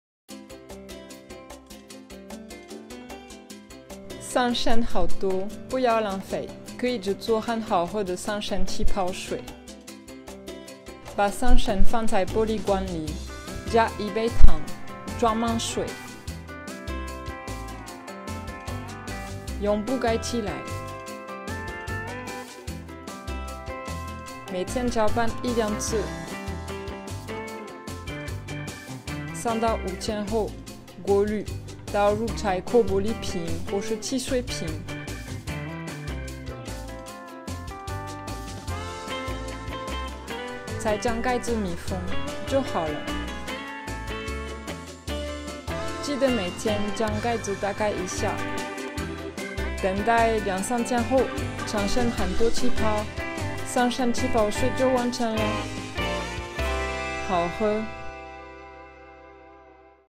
桑葚氣泡水DIY
跟克萊一起學如何自己做好喝的桑葚氣泡水吧！